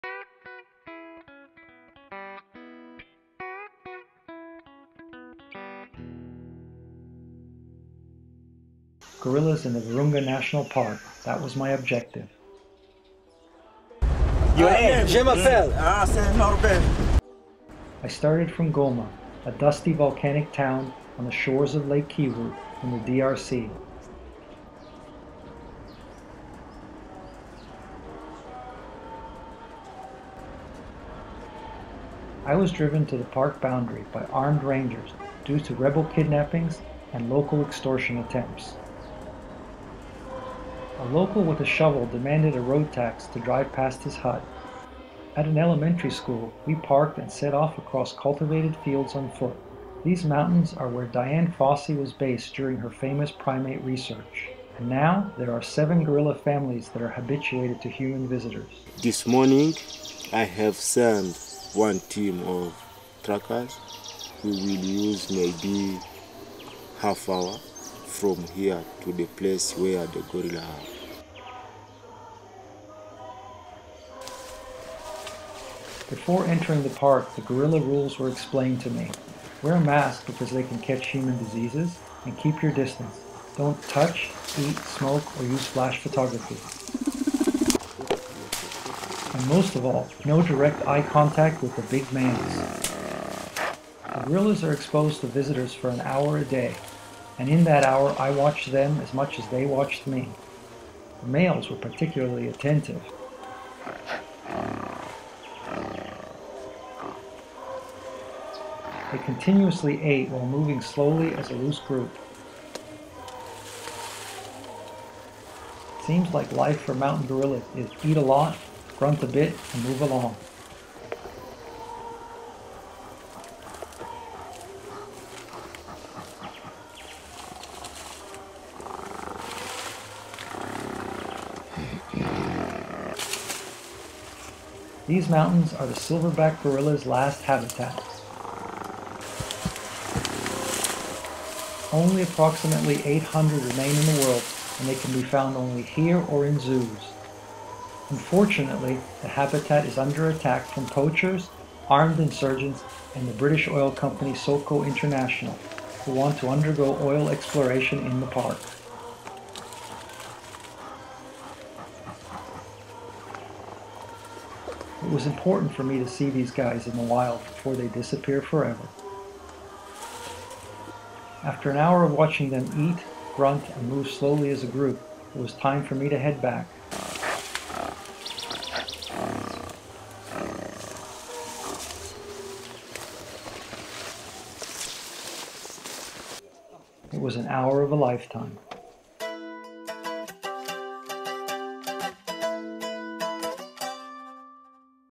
Africa, DRC Virunga Silverback Gorillas, Episode 126
The Silverback gorillas of the Virunga mountains are the last in the world (less than 800 per last count). After watching the Virunga documentary & reading about SOCO International I had to visit. Getting there was a bit complicated but doable for the brave of heart and persistent of paperwork. And with the DRC being such an unstable place you can count on some complications, even when I am escorted by  four armed rangers at all times. Life in the DRC is complicated, even for humans.